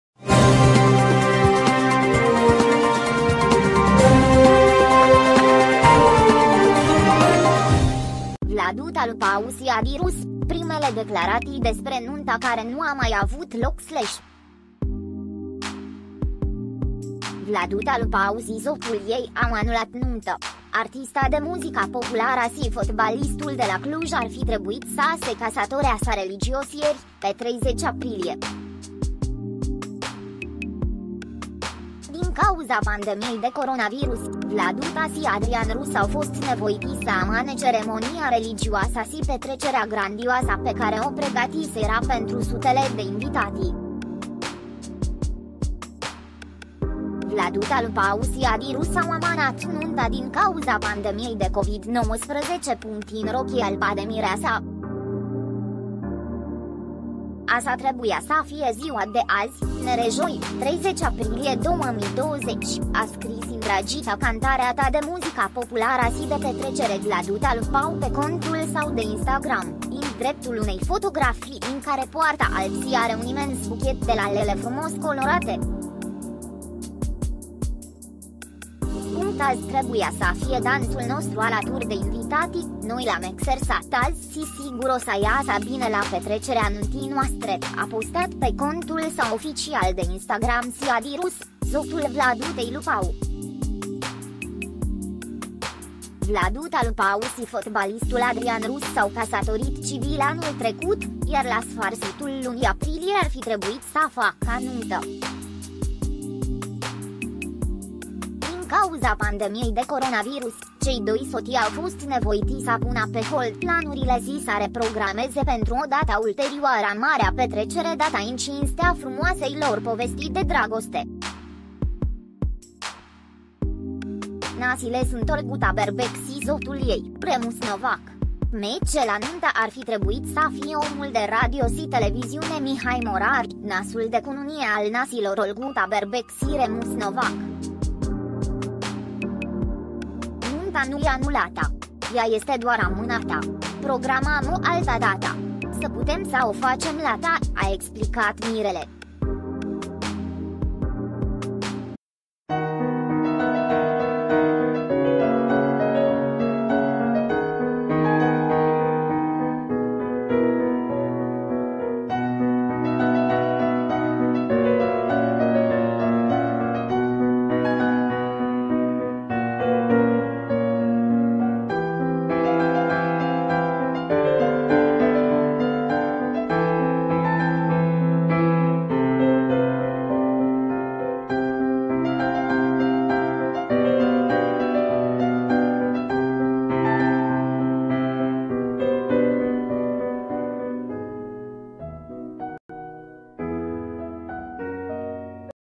Vladuta Lupau a anulat nunta! Afla motivul
Vladuta Lupau si Adi Rus, primele declaratii despre nunta care nu a mai avut loc/
Vladuta Lupau si sotul ei au anulat nunta. Artista de muzica populara si fotbalistul de la Cluj ar fi trebuit sa se casatoreasa religios ieri, pe 30 aprilie. Din cauza pandemiei de coronavirus, Vladuta si Adrian Rus au fost nevoiti sa amane ceremonia religioasa si petrecerea grandioasa pe care o pregatisera pentru sutele de invitati.

Vladuta Lupau si Adi Rus au amanat nunta din cauza pandemiei de COVID-19
"In rochie alba de mireasa... Asa trebuia sa fie ziua de azi (n.r. joi, 30 aprilie 2020)", a scris indragita cantareata de muzica populara si de petrecere Vladuta Lupau pe contul sau de Instagram, in dreptul unei fotografii in care poarta alb si are un imens buchet de lalele frumos colorate.

"Azi trebuia sa fie dansul nostru alãturi de invitati,noi l-am exersat azi si sigur o sa iasã bine la petrecerea nuntii noastre", a postat pe contul sau oficial de Instagram si Adi Rus, sotul Vladutei Lupau.

Vladuta Lupau si fotbalistul Adrian Rus s-au casatorit civil anul trecut, iar la sfarsitul lunii aprilie ar fi trebuit sa faca nunta. Din cauza pandemiei de coronavirus, cei doi soti au fost nevoiti sa puna pe hold planurile si sa reprogrameze pentru o data ulterioara marea petrecere data in cinstea frumoasei lor povesti de dragoste. Nasi le sunt Olguta Berbec si sotul ei, Remus Novac. MC la nunta ar fi trebuit sa fie omul de radio si televiziune Mihai Morar, nasul de cununie al nasilor Olguta Berbec si Remus Novac.

"Nunta nu e anulatã,
Ea este doar amânatã,
Programãm o altã datã,
Sã putem sã o facem latã", a explicat mirele.